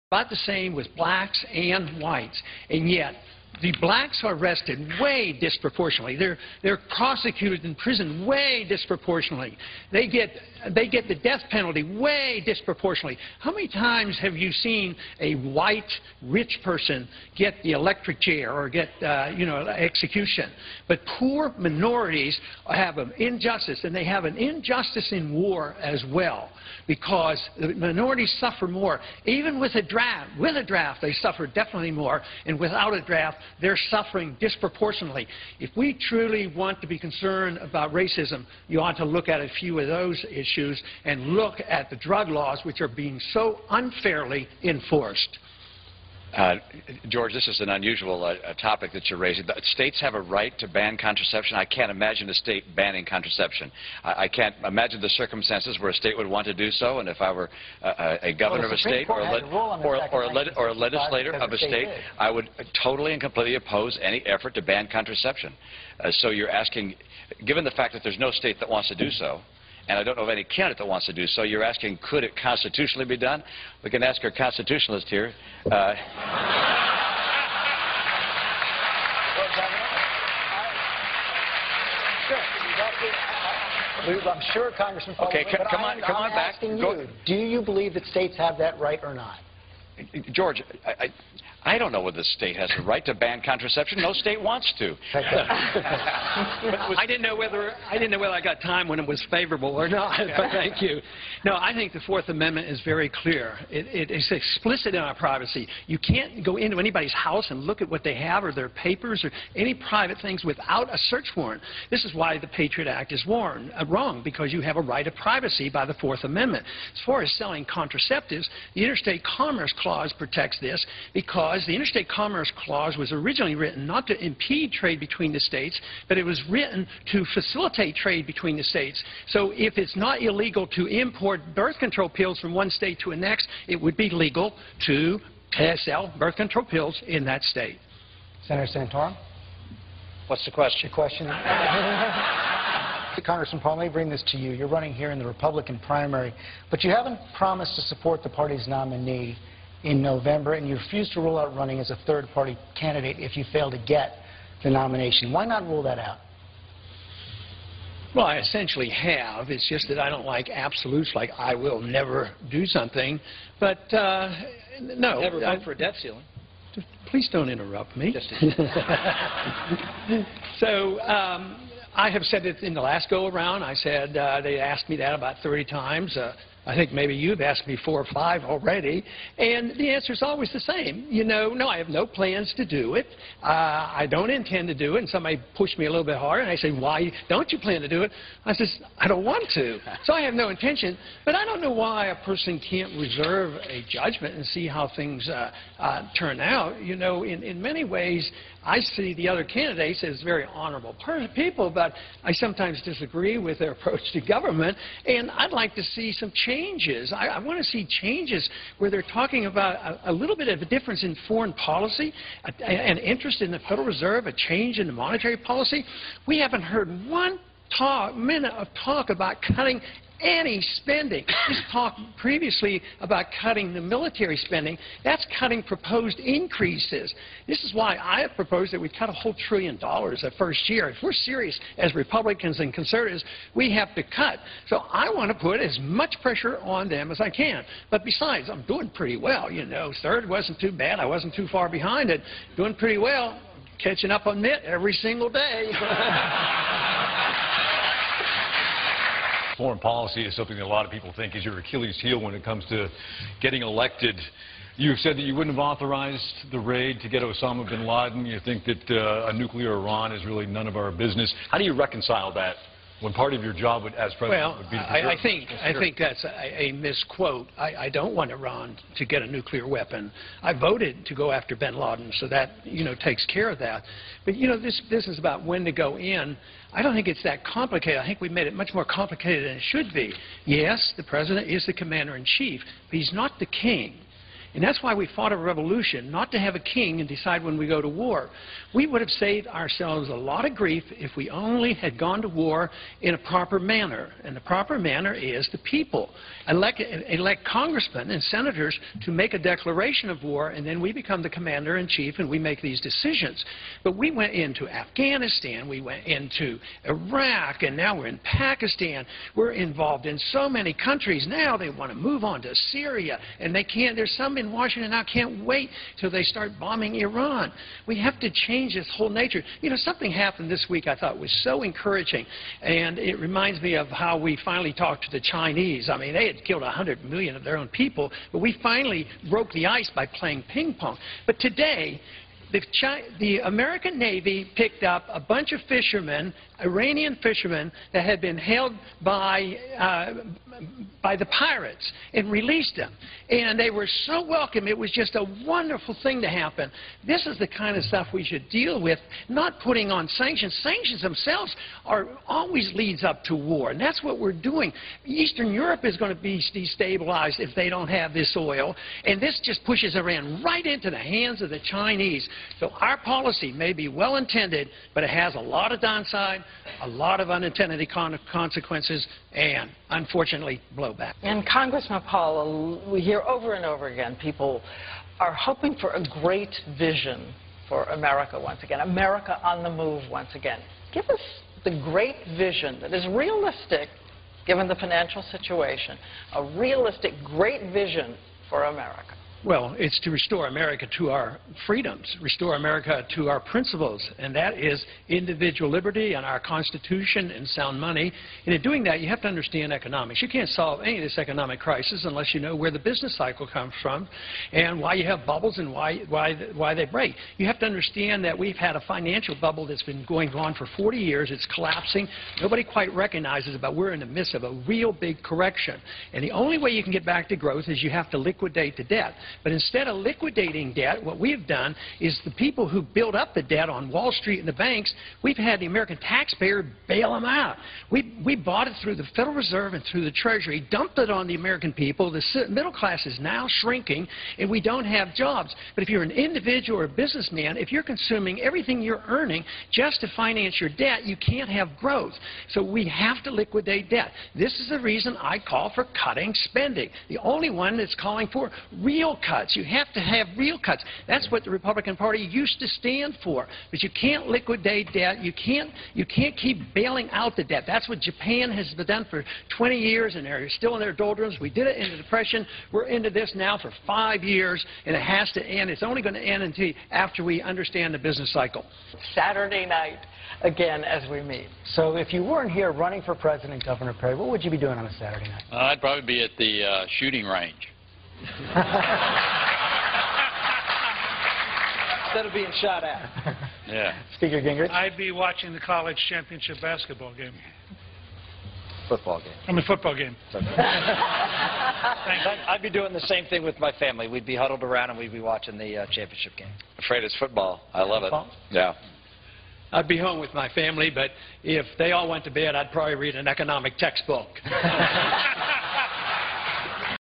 Ron Paul Highlights ∞ New Hampshire Debate (2/2) 1-7-12
Ron Paul continues to educate and mold the political debate
Ron Paul 2012